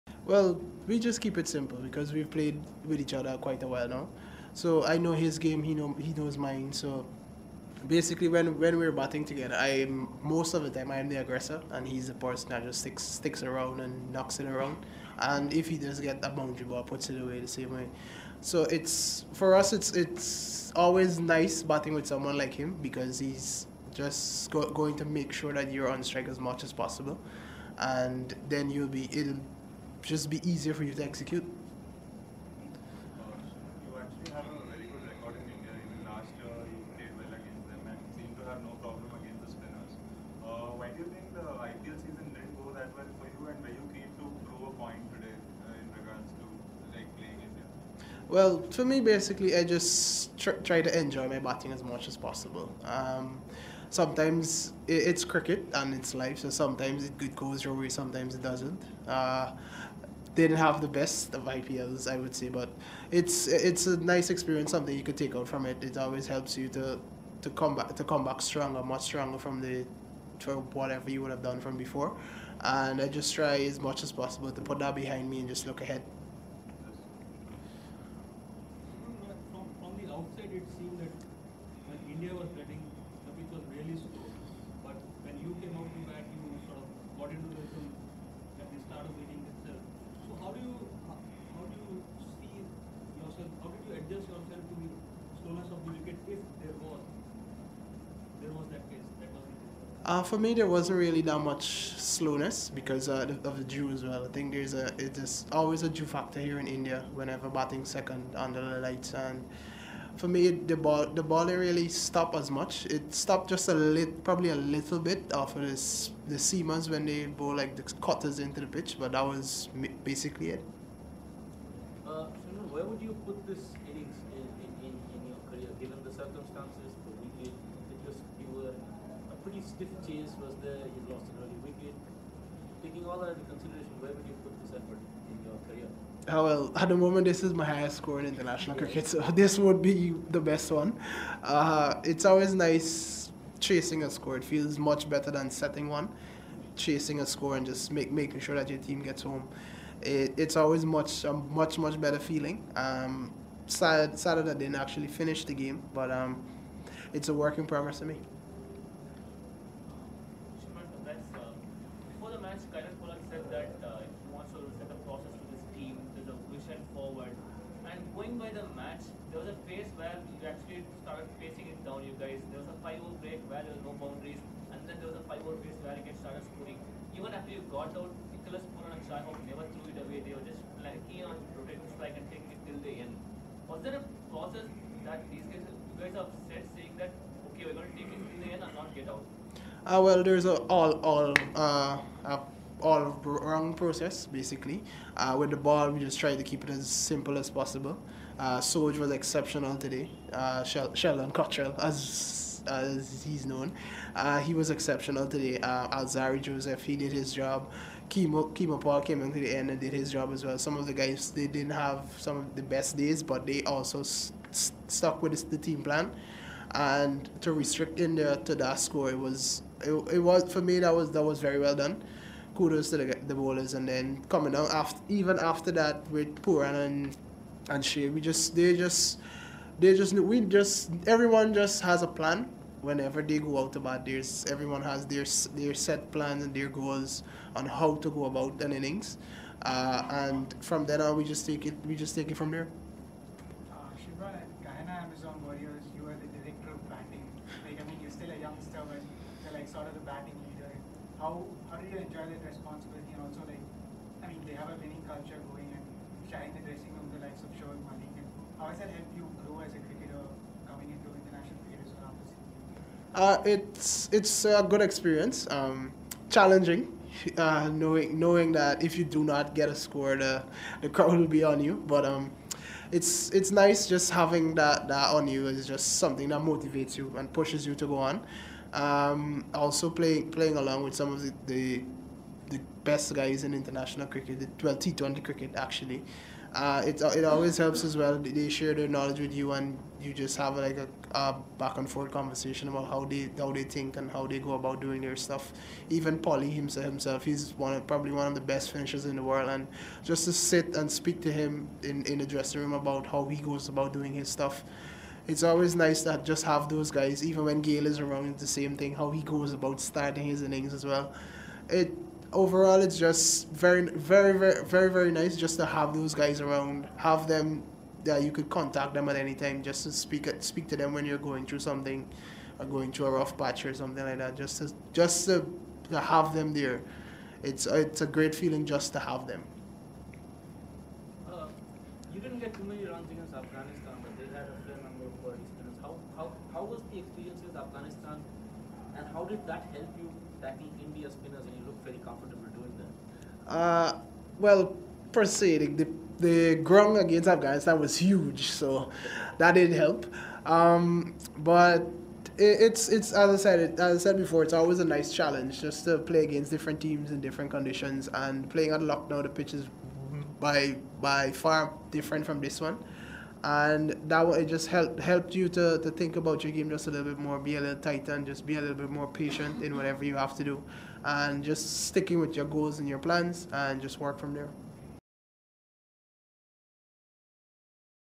Shimron Hetmyer on His 139 That Helped WI Win Series Opener vs India | The Quint
Centurion Shimron Hetmyer said his knock of 139 means a lot to him, especially since West Indies ended up on the winning side against India in the first ODI in Chennai.